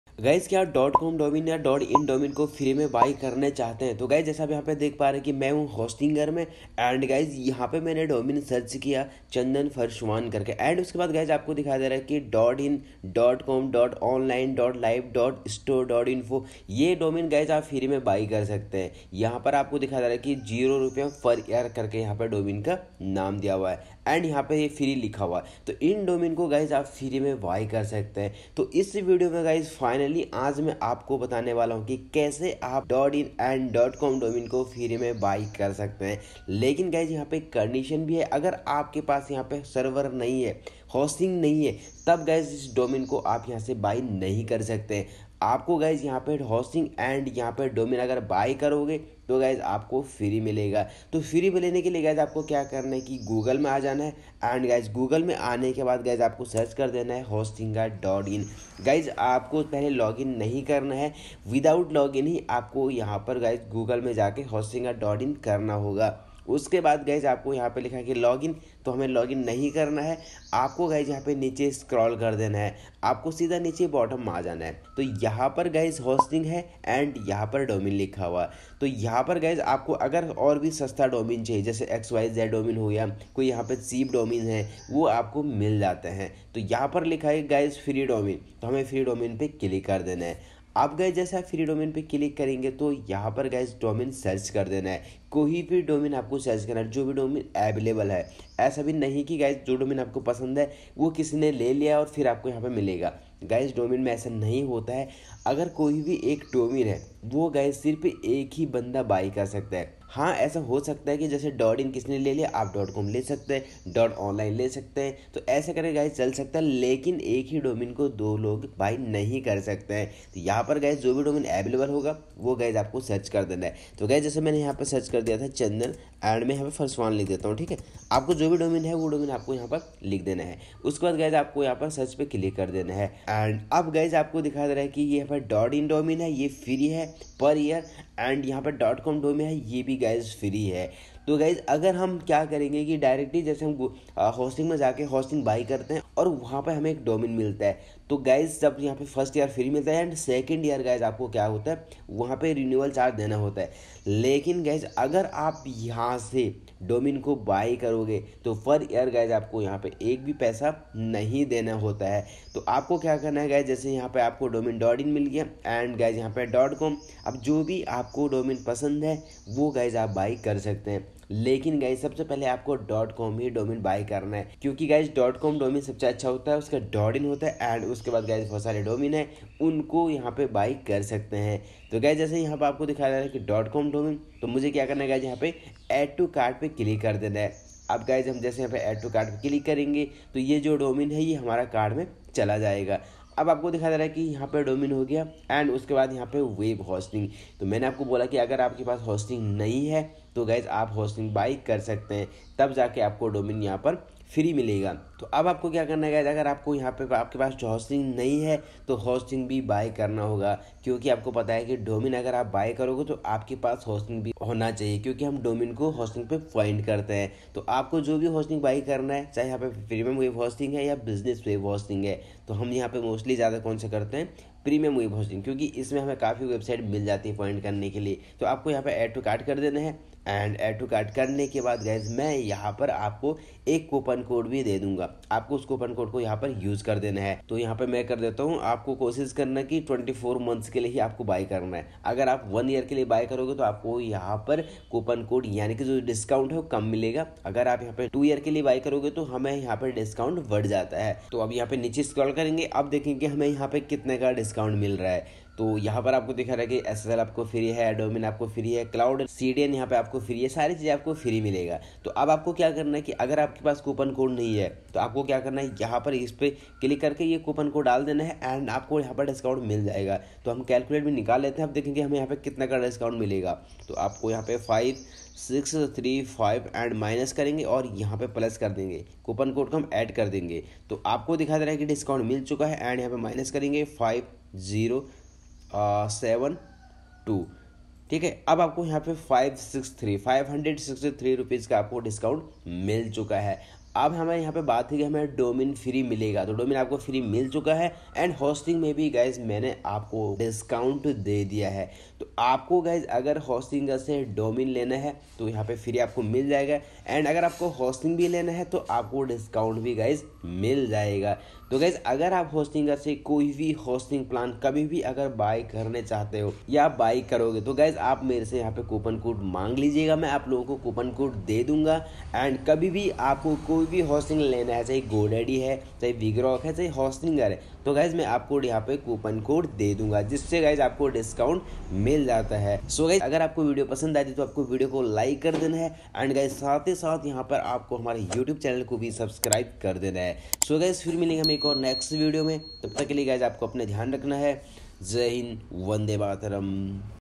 FREE .Com, .in Domain | How to Get Free Domain Name | Purchase free domain name in hostinger
FREE .Com, .in Domain | How to Get Free Domain Name | Purchase free domain name in hostinger | FREE HOSTING
dosto agr ap free domain buy krna chahte han to is video me humne btaya hai ki ap hostinger se free me .com and .in domain ko kaise buy kr sakte ho.

Thanks